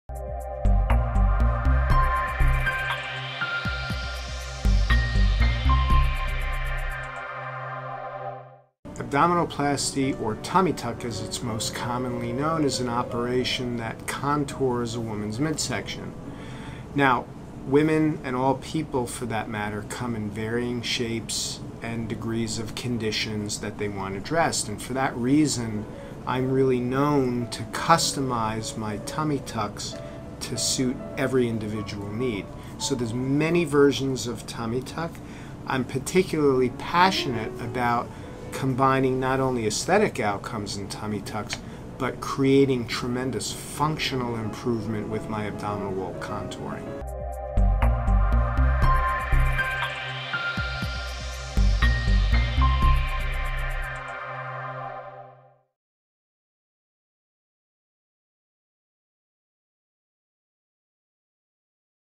Mommy Makeover | Spiro Plastic Surgery | Dr. Scott Spiro
Dr. Spiro, a board-certified expert plastic surgeon, explains a mommy makeover which includes a tummy tuck and breast procedure. Check out the beautiful results of Spiro Plastic Surgery

Dr. Scott Spiro | Spiro Plastic Surgery